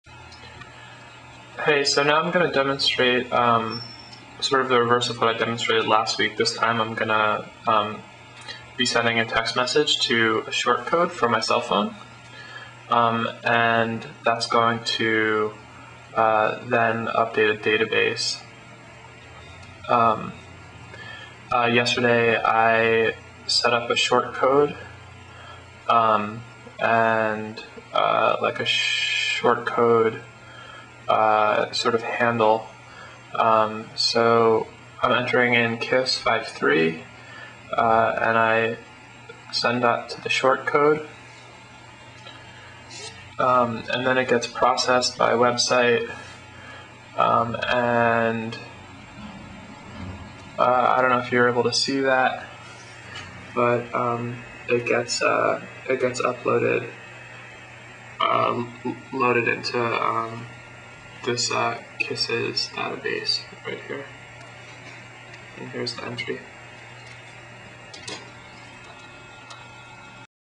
SMS to MYSQL
In this video, I demonstrate storing sms data in a mysql database.